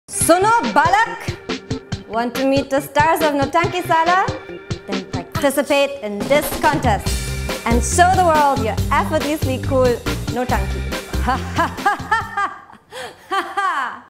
Evelyn Sharma's Nautanki
Dramebaaz Evelyn Sharma is eager to meet India's best Nautankis. Have you done your nautanki yet?